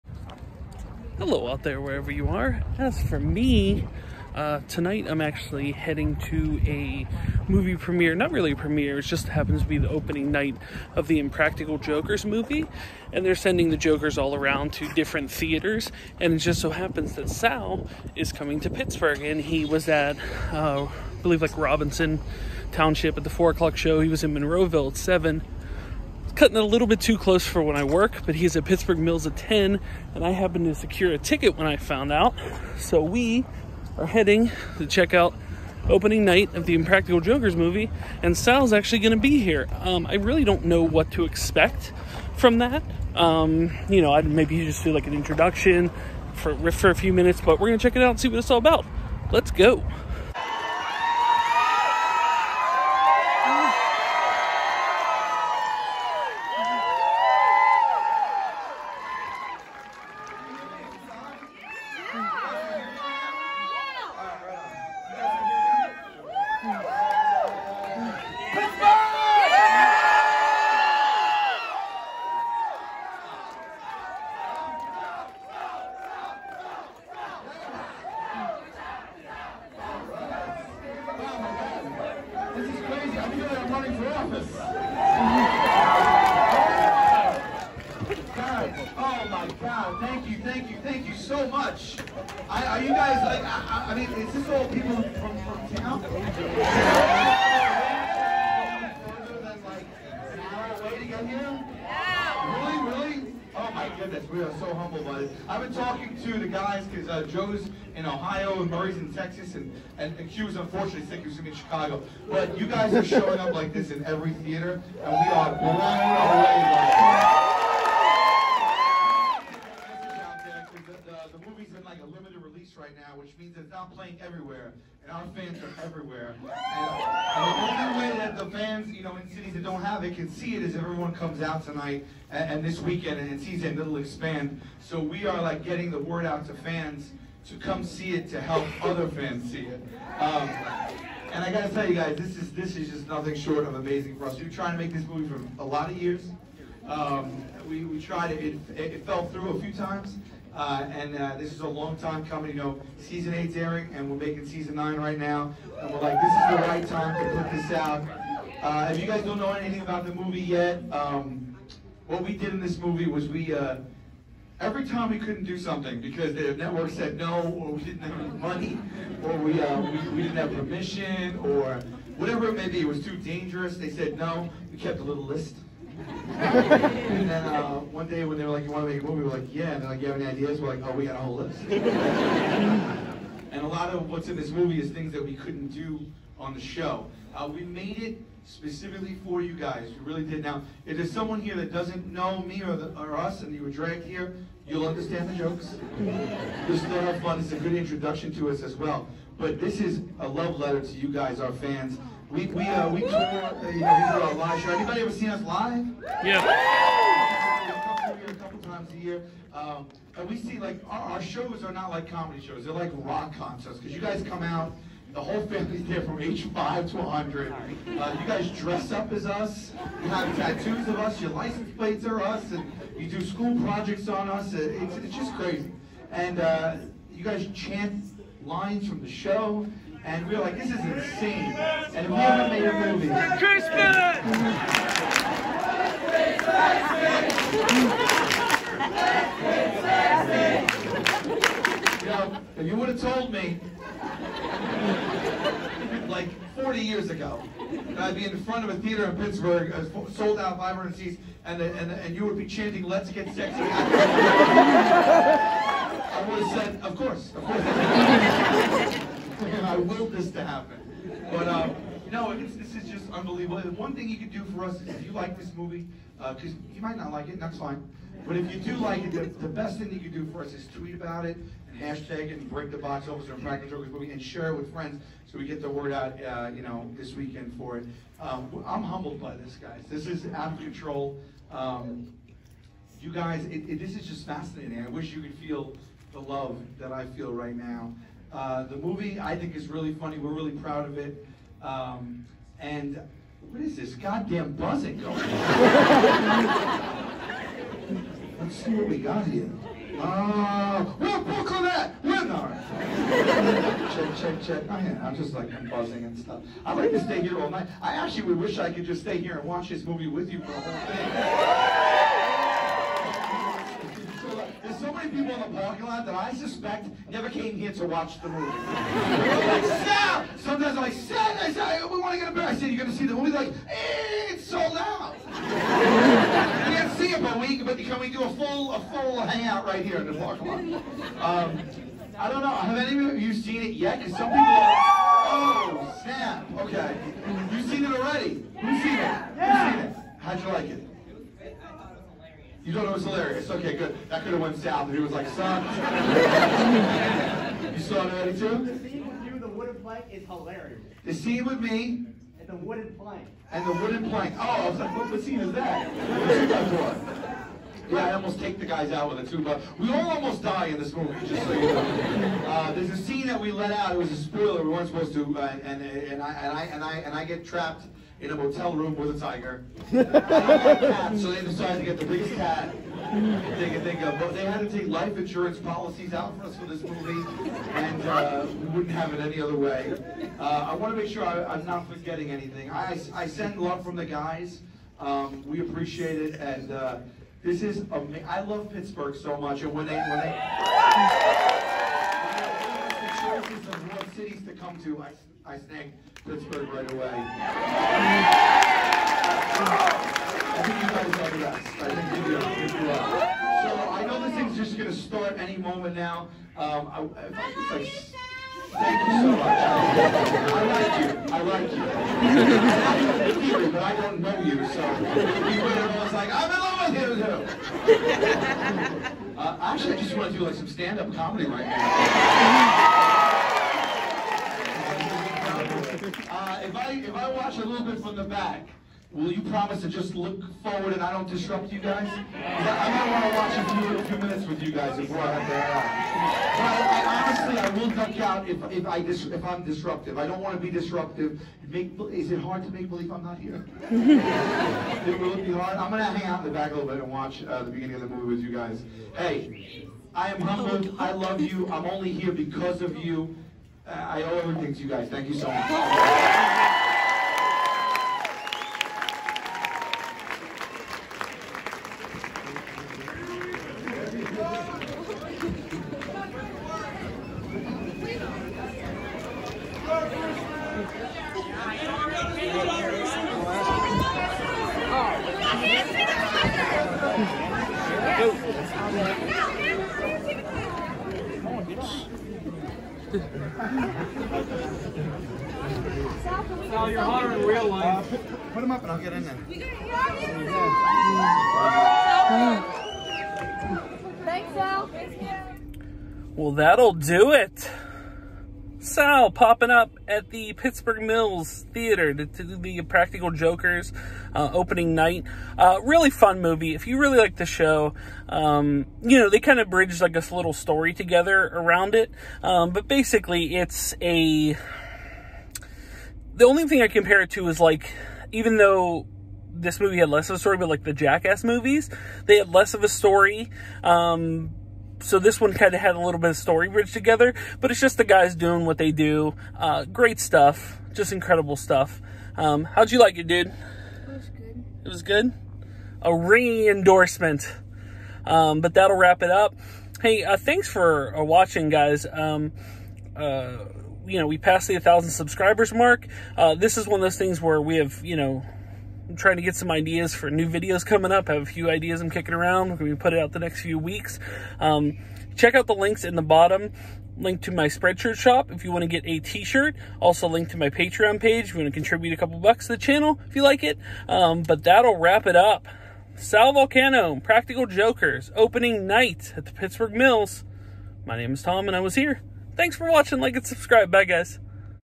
Tom Was Here - Sal Vulcano at Impractical Jokers Movie Opening Night - Pittsburgh, PA - Feb. 2020
The Impractical Jokers movie opened on Friday and I just happen to stumble upon a post on social media that they were sending the Jokers out to various theaters on opening weekend to increase some buzz for the movie while its in limited release. It just so happened that Sal Vulcano was spending the Opening night for the film in Pittsburgh. Luckily I found out early enough that I was still able to get tickets for the 10pm show at Pittsburgh Mills. Sal has always shown a great affection for the city of Pittsburgh as well as the Steelers. The movie is great, it’s the guys doing what they do on a much larger scale. Great movie.